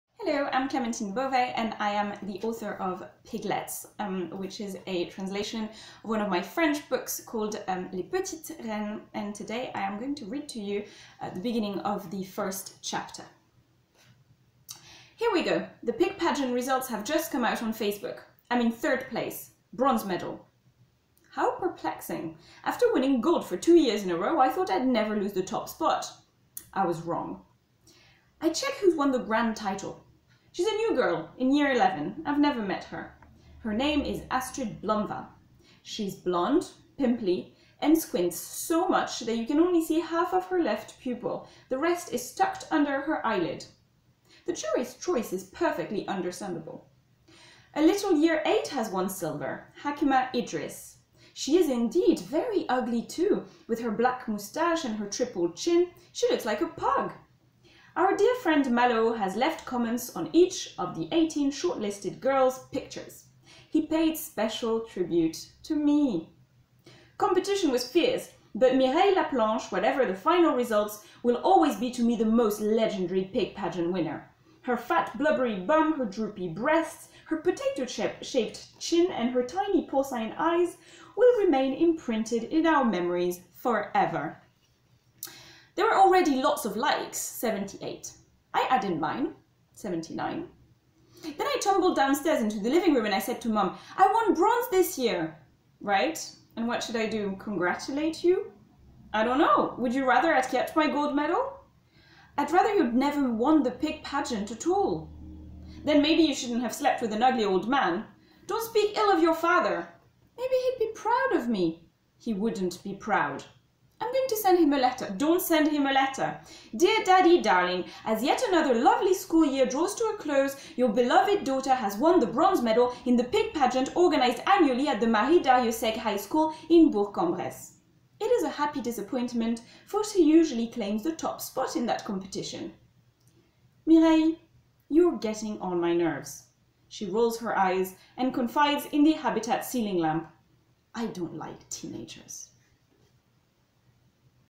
Clémentine Beauvais reads from Piglettes!
The brilliant Piglettes by Clémentine Beauvais is out for the first time in English on July 6th 2017! Already a huge bestseller in France, this soon-to-be YA classic will steal your heart - listen to author Clémentine reading the first chapter.